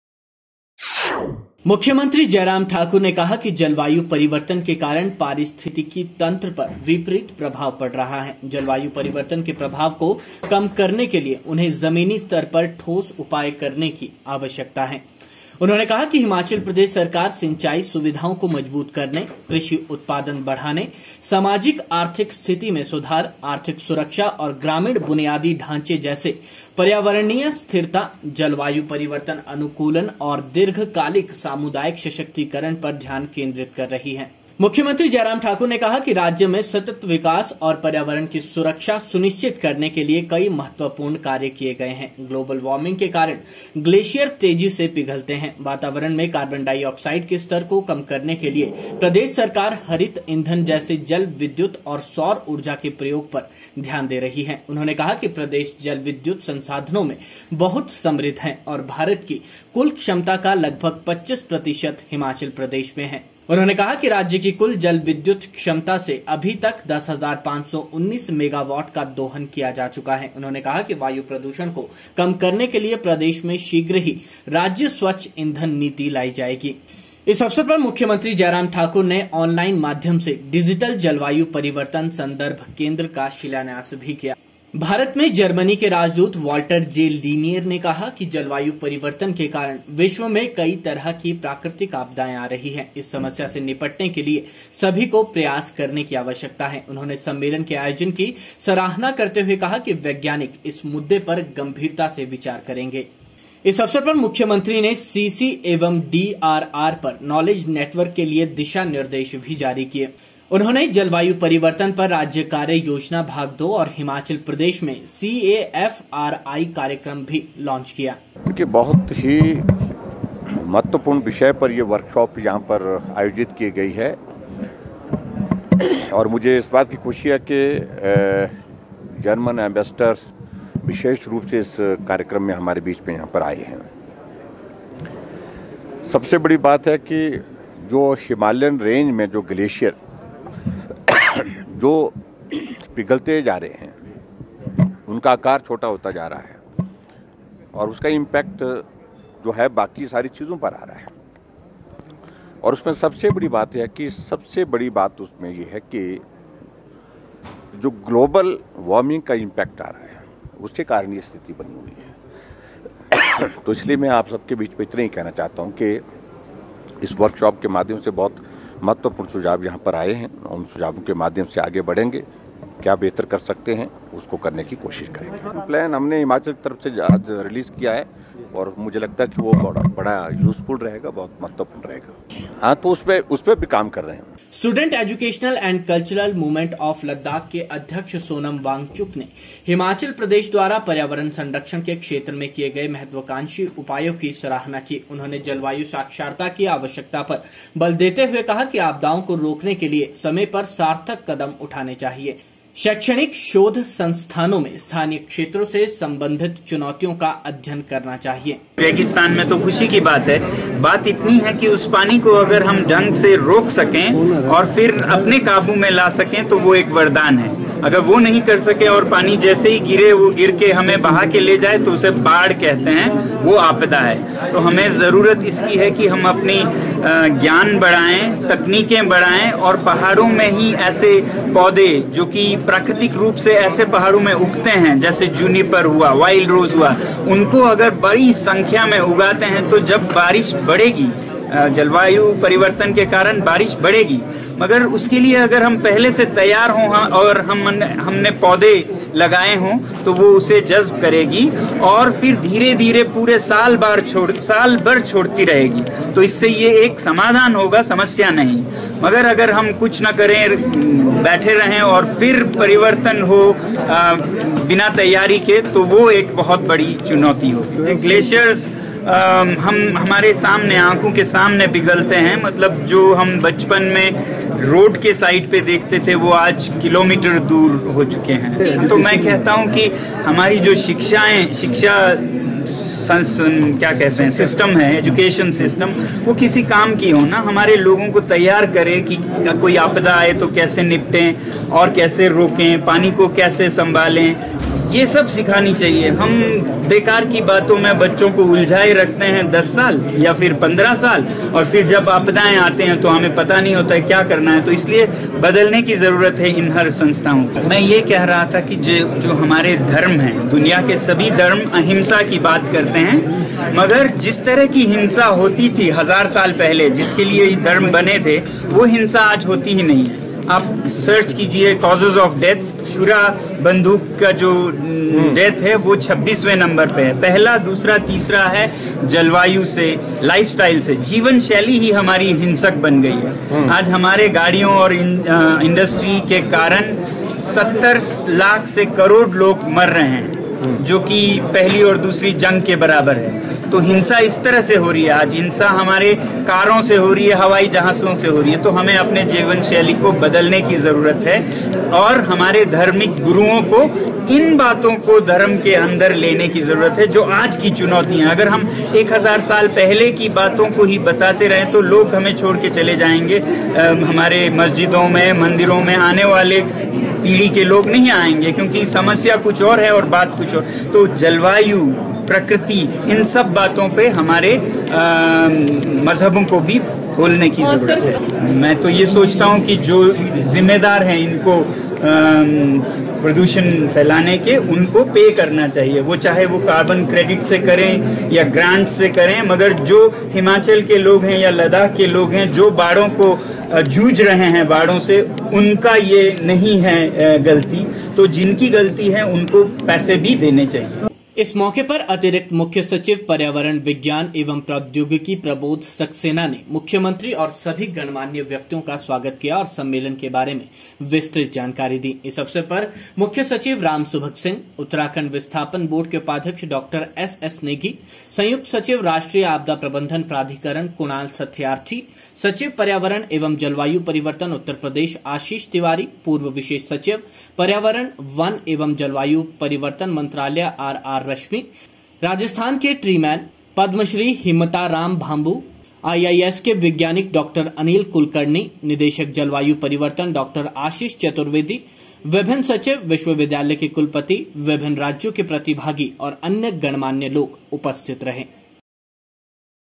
मुख्यमंत्री ने जलवायु परिर्वतन के प्रभाव को कम करने के लिए ठोस उपाय करने पर बल दिया
मुख्यमंत्री जय राम ठाकुर ने कहा कि प्रगति और प्रकृति के  बीच सामंजस्य बनाकर ही जलवायु परिर्वतन से सम्बन्धित चुनौतियों से प्रभावी तरीके से निपटा जा सकता है। यह बात मुख्यमंत्री ने आज यहां पर्यावरण, विज्ञान एवं प्रौद्योगिकी विभाग द्वारा आयोजित सुदृढ़ हिमालय सुरक्षित भारत, जलवायु परिवर्तन सम्मेलन-2021 की अध्यक्षता करते हुए कही।मुख्यमंत्री ने कहा कि जलवायु परिवर्तन के कारण पारिस्थितिकी तंत्र पर विपरित प्रभाव पड़ रहा है। जलवायु परिवर्तन के प्रभाव को कम करने के लिए हमें जमीनी स्तर पर ठोस उपाय करने की आवश्यकता है। उन्होंने कहा कि हिमाचल प्रदेश सरकार सिंचाई सुविधाओं को मजबूत करने, कृषि उत्पादन बढ़ाने, सामाजिक-आर्थिक स्थिति में सुधार, आर्थिक सुरक्षा और ग्रामीण बुनियादी ढांचे जैसे पर्यावरणीय स्थिरता, जलवायु परिर्वतन अनुकुलन और दीर्घकालिक सामुदायिक सशक्तिकरण पर ध्यान केन्द्रित कर रही है। जय राम ठाकुर ने कहा कि राज्य में सत्त विकास और पर्यावरण की सुरक्षा सुनिश्चित करने के लिए कई महत्वपूर्ण कार्य किए हैं। ग्लोबल वार्मिंग के कारण ग्लेशियर तेजी से पिघलते हैं। वातावरण में कार्बनडाइआॅक्साइड के स्तर को कम करने के लिए प्रदेश सरकार हरित ईंधन जैसे जल विद्युत और सौर उर्जा के प्रयोग पर ध्यान दे रही है। उन्होंने कहा कि प्रदेश जल विद्युत संसाधनों में बहुत समृद्ध है और भारत की कुल क्षमता का लगभग 25 प्रतिशत हिमाचल प्रदेश में है। राज्य की कुल जल विद्युत क्षमता में से अभी तक 10,519 मेगावाट का दोहन किया जा चुका है। उन्होंने कहा कि वायु प्रदूषण को कम करने के लिए प्रदेश में शीघ्र ही राज्य स्वच्छ ईंधन नीति लाई जाएगी। जय राम ठाकुर ने कहा कि जलवायु परिर्वतन की समस्या से निपटने के लिए सामूहिक प्रयास करने की आवश्यकता है। हमें सार्थक ग्लोबल वार्मिंग कानून का समर्थन करना होगा तथा बिजली संयंत्रों की ऊर्जा दक्षता में सुधार के साथ-साथ नवीकरणीय ऊर्जा स्रतों का उपयोग भी बढ़ाना होगा। इस अवसर पर मुख्यमंत्री ने आॅनलाइन माध्यम से डिजिटल जलवायु परिर्वतन सन्दर्भ केन्द्र का शिलान्यास भी किया। भारत में जर्मनी के राजदूत वाल्टर जे. लीनेयर ने कहा कि जलवायु परिवर्तन के कारण विश्व में कई तरह की प्राकृतिक आपदाएं आ रही हैं। इस समस्या से निपटने के लिए सभी को प्रयास करने की आवश्यकता है। उन्होंने सम्मेलन के आयोजन की सराहना करते हुए कहा कि वैज्ञानिक इस मुददे पर गम्भीरता से विचार करेंगे। इस अवसर पर मुख्यमंत्री ने सीसी एवं डीआरआर पर नाॅलेज नेटवर्क के लिए दिशा-निर्देश भी जारी किए। उन्होंने जलवायु परिवर्तन पर राज्य कार्य योजना भाग-2 और हिमाचल प्रदेश में सीएएफआरआई कार्यक्रम भी लाॅंच किया।
स्टूडेंट एजुकेशनल एण्ड कल्चरल मूमेंट आॅफ लद्दाख के अध्यक्ष सोनम वांगचुक ने हिमाचल प्रदेश द्वारा पर्यावरण संरक्षण के क्षेत्र में किए गए महत्वाकांक्षी उपायों की सराहना की। उन्होंने जलवायु साक्षरता की आवश्यकता पर बल देते हुए कहा कि आपदाओं को रोकने के लिए समय पर सार्थक कदम उठाए जाने चाहिए। शैक्षणिक शोध संस्थानों में स्थानीय क्षेत्रों से सम्बन्धित चुनौतियों का अध्ययन किया जाना चाहिए।
राष्ट्रीय आपदा प्रबन्धन प्राधिकरण के महानिदेशक कमल किशोर ने कहा कि हर क्षेत्र की आपदा प्रबन्धन योजना बनाई जानी चाहिए। उन्होंने सभी राज्यों में राज्य आपदा प्रतिक्रिया बल के गठन की आवश्यकता पर बल दिया।वरिष्ठ सलाहकार विज्ञान और प्रौद्योगिकी विभाग भारत सरकार डाॅ. अखिलेश गुप्ता ने कहा कि ग्लोबल वार्मिंग विश्वभर में एक बड़ी चुनौती है। उन्होंने बहु जोखिम चेतावनी प्रणाली तंत्र विकसित करने की आवश्यकता पर बल दिया। अतिरिक्त मुख्य सचिव पर्यावरण विज्ञान एवं प्रौद्योगिकी प्रबोध सक्सेना ने मुख्यमंत्री और सभी गणमान्य व्यक्तियों का स्वागत किया और सम्मेलन के बारे में विस्तृत जानकारी दी। निदेशक पर्यावरण, विज्ञान एवं प्रौद्योगिकी सुदेश कुमार मोकटा ने धन्यवाद प्रस्ताव प्रस्तुत किया। इस अवसर पर मुख्य सचिव राम सुभग सिंह, उत्तराखण्ड विस्थापन बोर्ड के उपाध्यक्ष डाॅ. एसएस नेगी, संयुक्त सचिव राष्ट्रीय आपदा प्रबन्धन प्राधिकरण कुणाल सत्यार्थी, सचिव पर्यावरण एवं जलवायु परिवर्तन उत्तर प्रदेश आशीष तिवारी, पूर्व विशेष सचिव, पर्यावरण, वन एवं जलवायु परिवर्तन मंत्रालय आरआर रश्मी, राजस्थान के ट्री-मैन, पदमश्री हिम्मताराम भांभू, आईआईएस के वैज्ञानिक डाॅ. अनिल कुलकर्णी, निदेशक जलवायु परिवर्तन डाॅ. आशीष चतुर्वेदी, विभिन्न सचिव, विश्वविद्यालय के कुलपति, विभिन्न राज्यों के प्रतिभागी  और अन्य गणमान्य लोग उपस्थित थे।